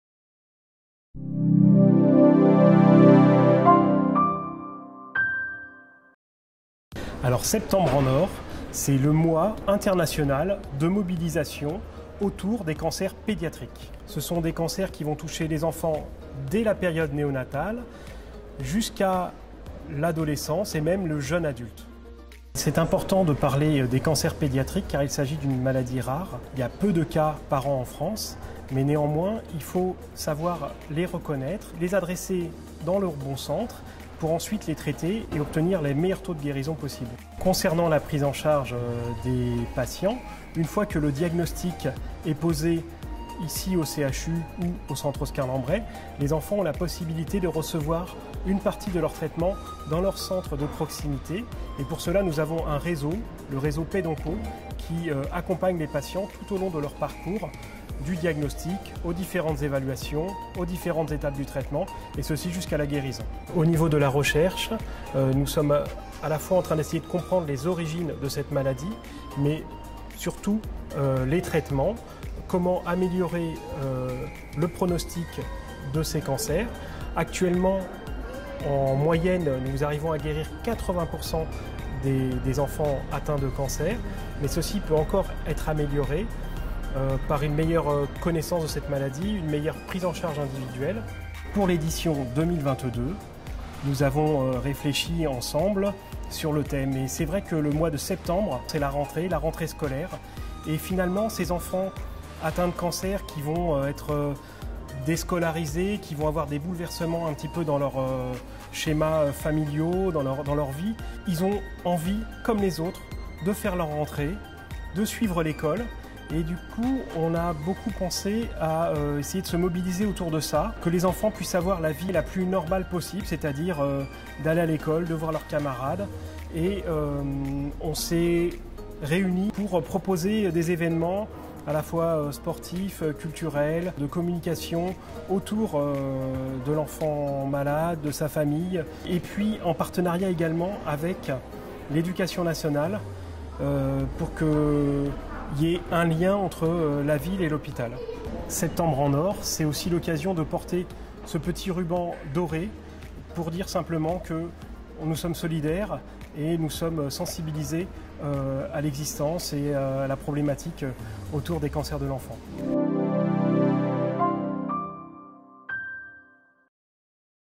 Septembre en Or - Le mois international des cancers de l'enfant
Le Dr. Abouchahla vous présente l'édition 2022 de Septembre en Or, le mois international des cancers de l'enfant.

Pendant tout le mois de septembre, au travers de différentes actions, le grand public est sensibilisé sur les cancers pédiatriques, leurs spécificités dans le diagnostic et leur prise en charge.